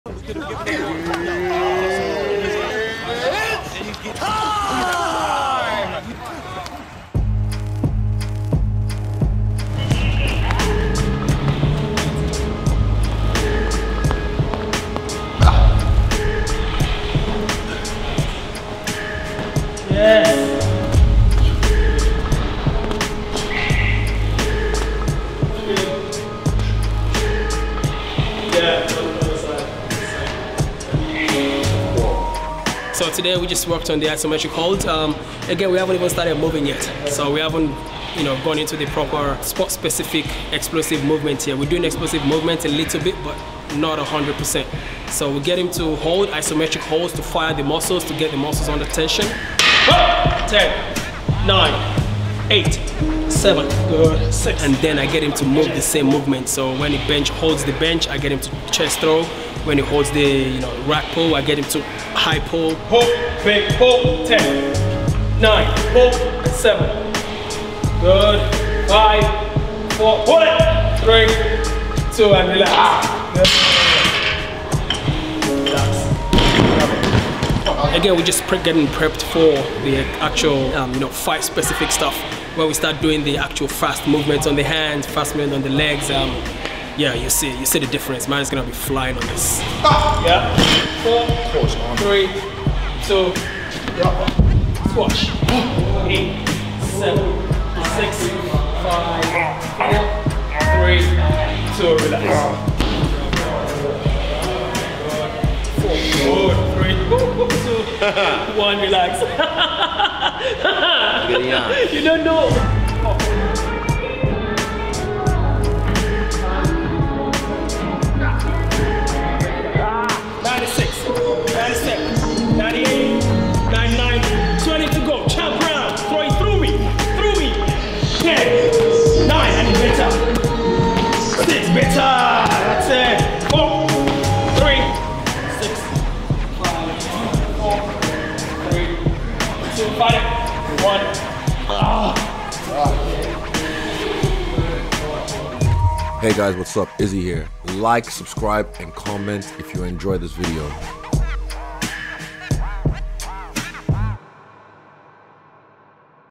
Inside Israel Adesanya's Strength & Conditioning Training for UFC 281
UFC Middleweight Champion Israel 'The Last Stylebender' Adesanya resumes his strength & conditioning training for his upcoming bout against Alex 'Poatan' Pereira, Live from Madison Square Garden in New York City on Saturday November 12.

Edited by: @StayHYDRATEDVisuals
Shot by: @DAVEBLAKAMOTO